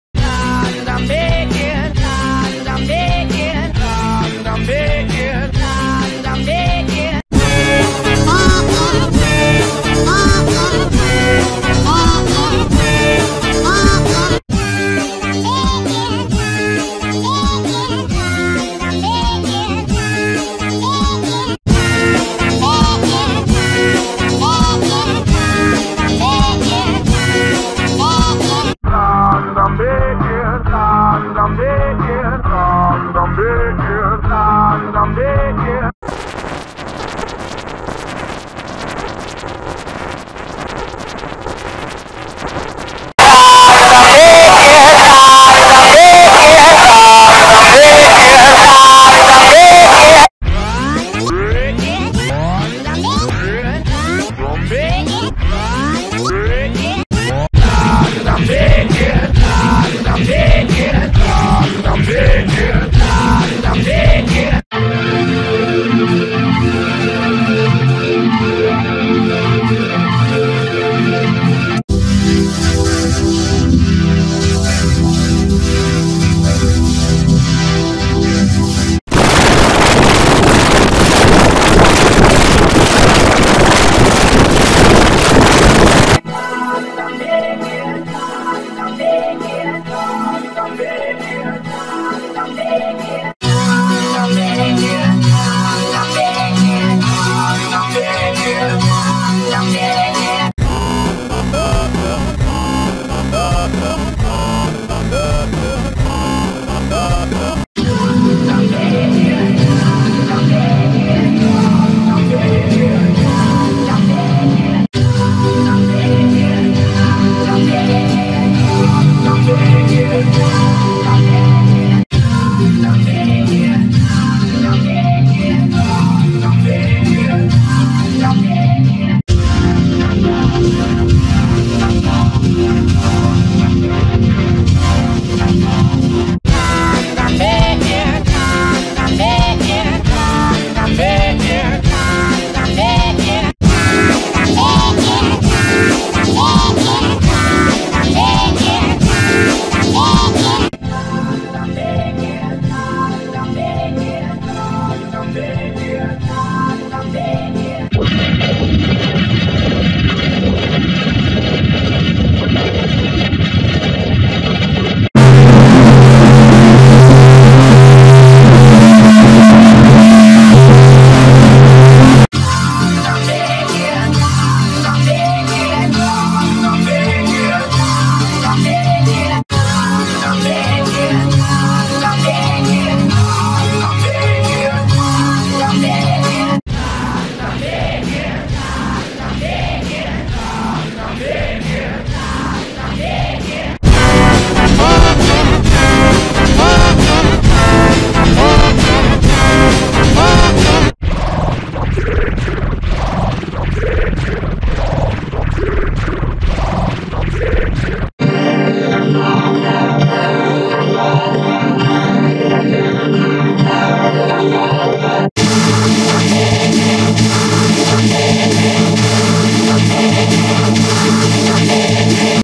Preview 2 Pear V2 Effects (My Sixth Preview)
Credit to Victor Coding for the source.

Effects List:
0. Normal
1. G Major 74
2. G Major 1
3. A Major
4. Fat X
5. Do Not Going X Is
6. Ear Bleed
7. X is High
8. O Major
9. Videoup v8
10. TailsCityNight
11. 4.0 DUH
12. NBK Full Chord
13. Shin Go Pro Chorded
14. X Has A Gamecrush
15. PowerYellowInvertFat
16. PowerLightBlueInvertFat
17. PowerTurquoiseInvertFat
18. BluePower
19. Glowshafted X
20. G Major 29
21. Fmaj Vocoder 2.0
22. 34.0 DUH
23. Chorded VHS
24. Cloudy Charm Chorded
25. Monoround Chorded
26. Blue Skies
27. Khord
28. Grayscale Large
29. Ensemble Effect 5.1
30. Supersaw Effect 3.0